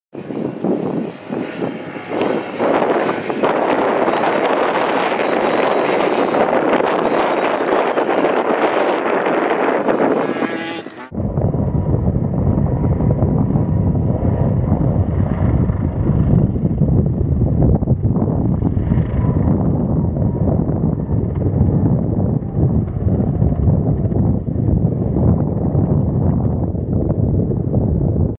RZR 4 Jump.mov
Polaris RZR 4 Robby Gordon Edition Jumping a dune at Winchester Bay Dunes on the Oregon Coast.  Tyler and Kyle are in the back along for the ride!